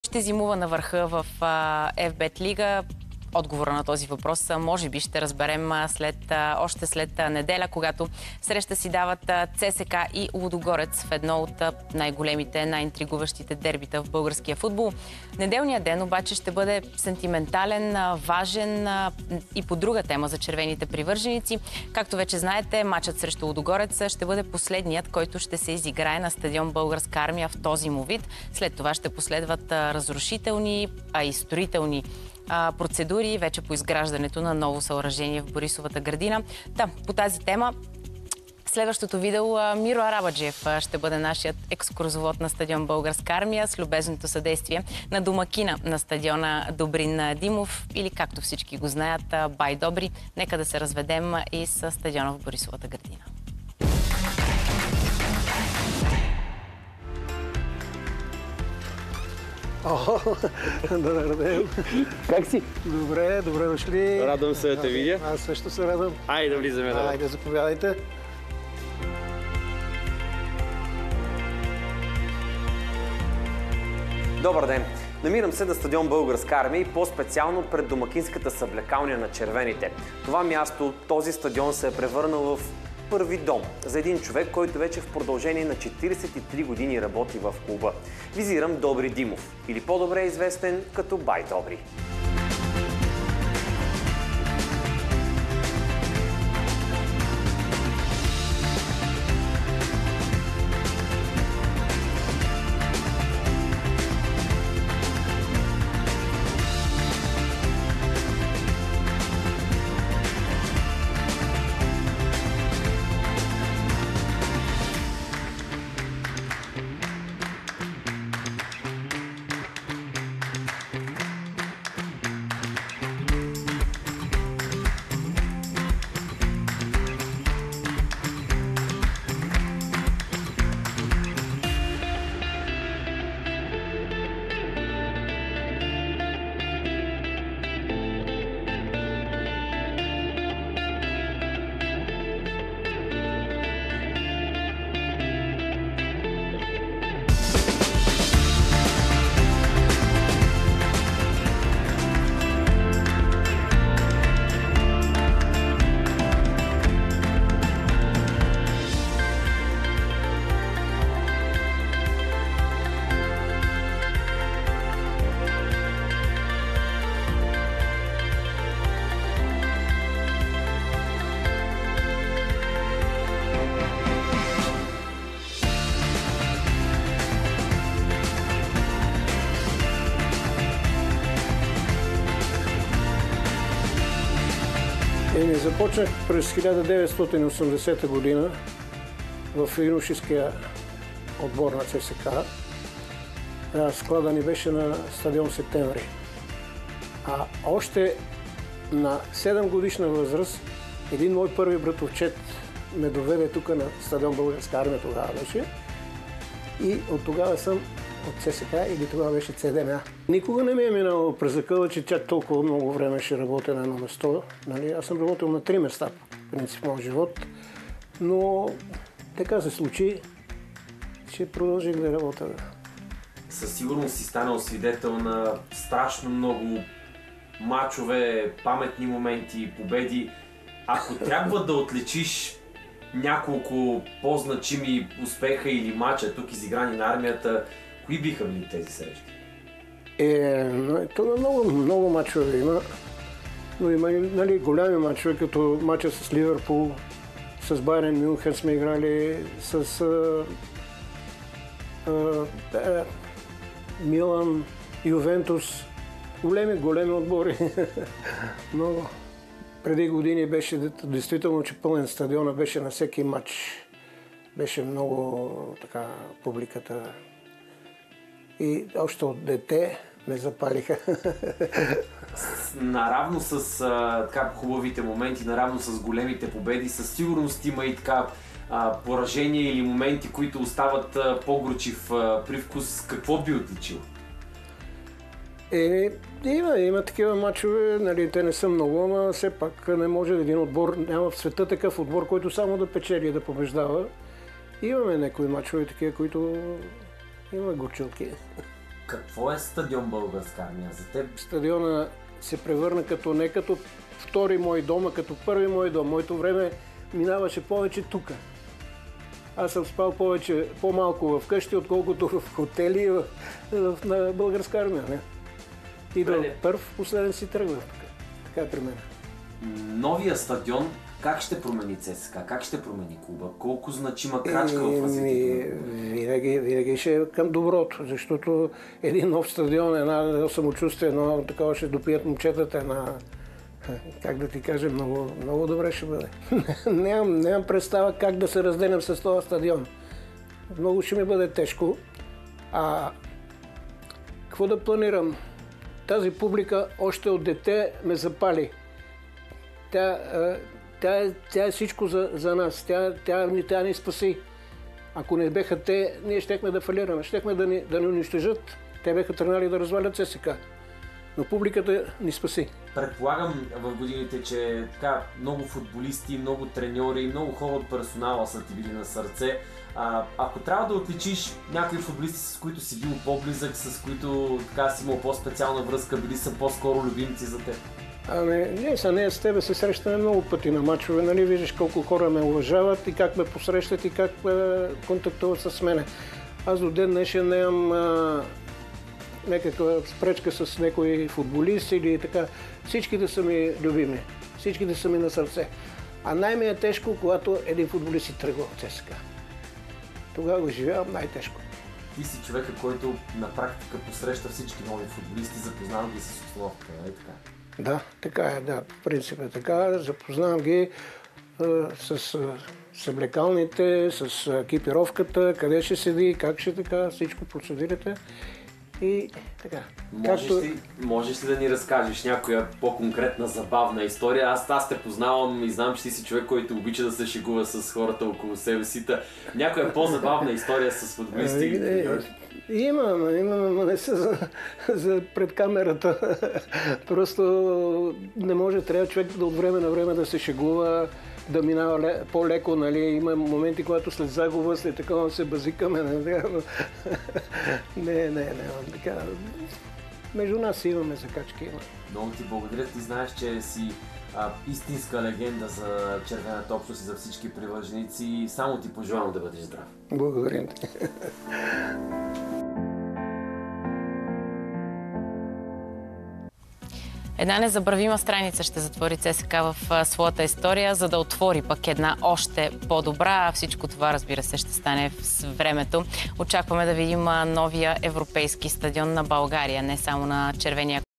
Домакинът на "Армията": Не знам как ще се разделя със стадиона, публиката спаси ЦСКА
Стадион "Българска армия" ще приеме последния мач в си този вид след няколко дни срещу Лудогорец. Спортният журналист Мирослав Арабаджиев ни развежда из "Армията" и ни запознава с домакина на стадиона Добрин Димов или по-добре познат като Бай Добри.